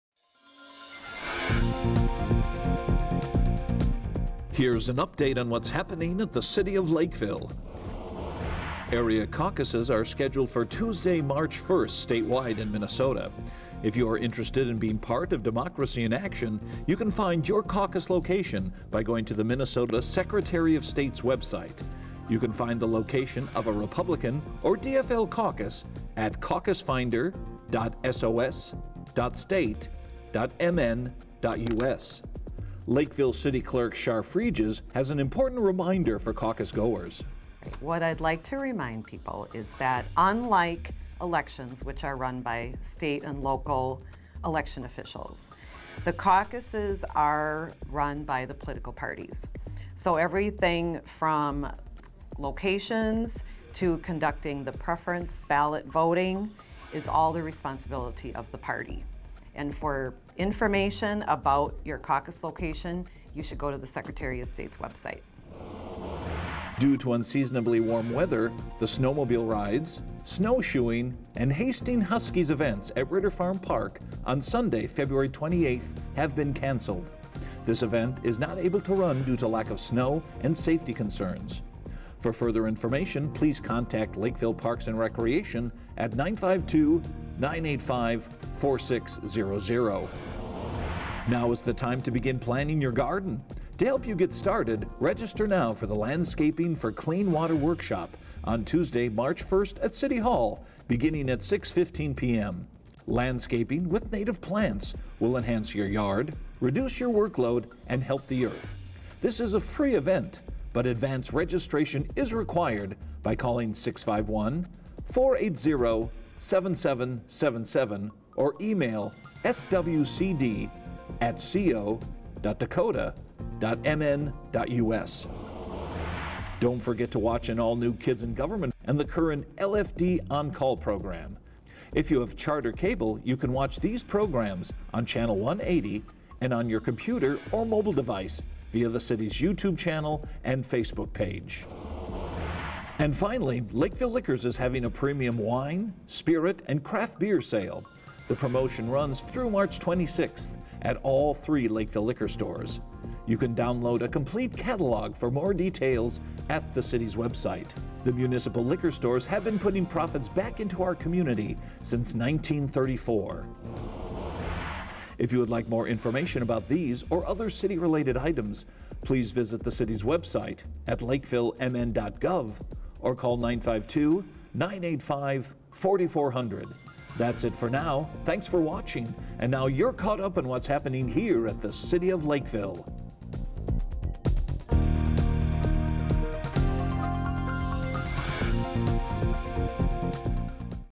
Focus on Lakeville 02-26-2016
AN update of news and events from the city of Lakeville, MN.